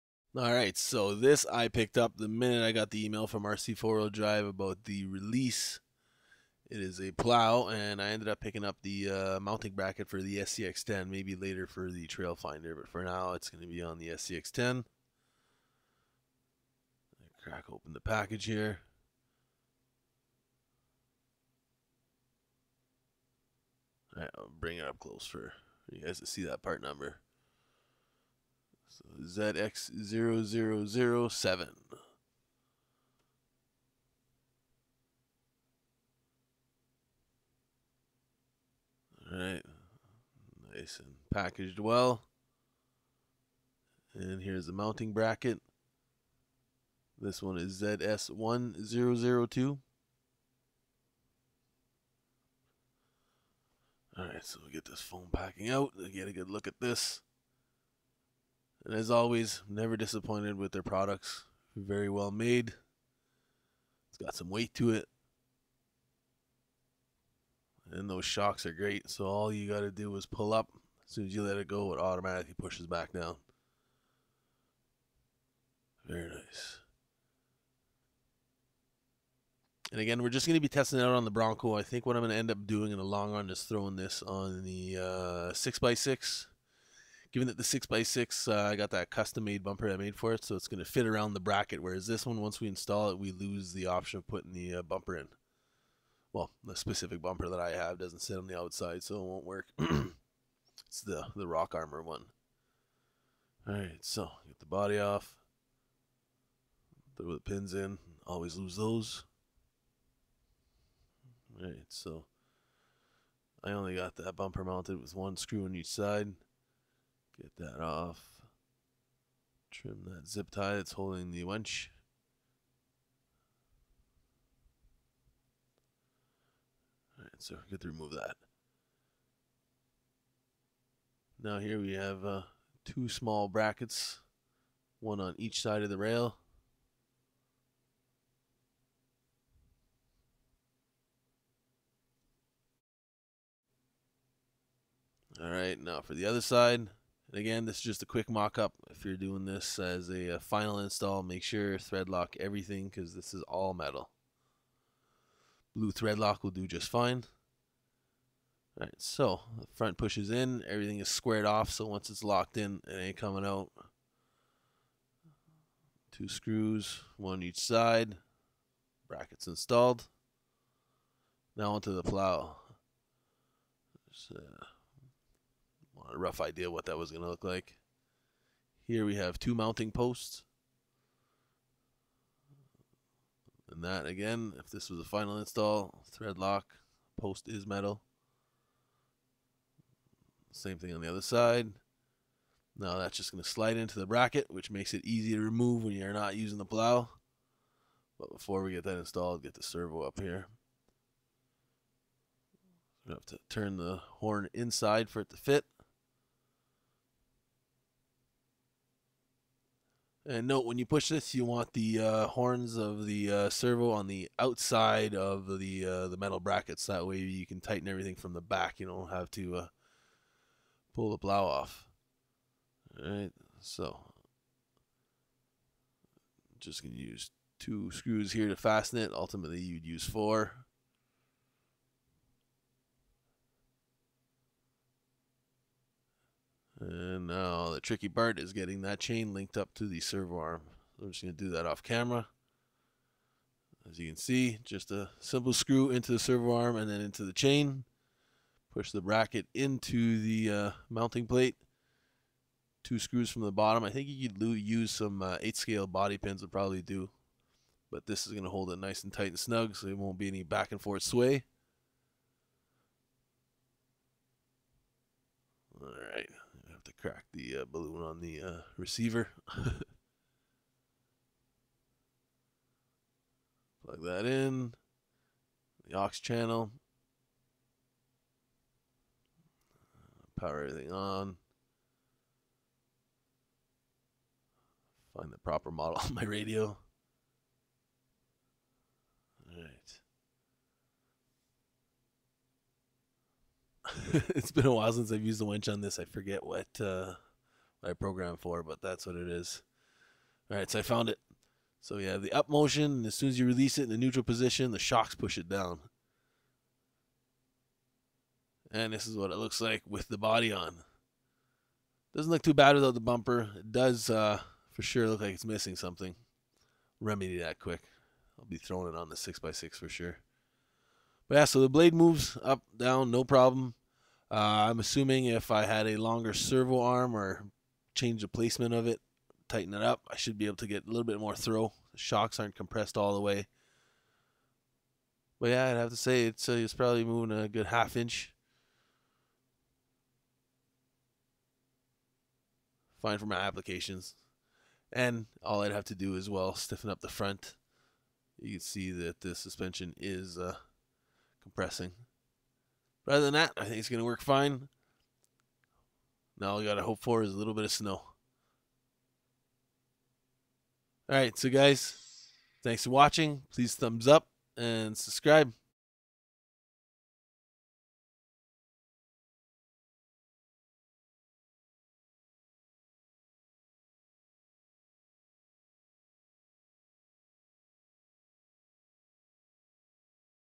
Installing The New RC4WD Blade Snow Plow on my Axial SCX10
It's finally here and all I'm missing is snow. The instal was very straight forward. No issues. The build quality is great. All I need to do is shorten the chain going to the plow and it will lift much higher. Any questions or comments please leave them below,.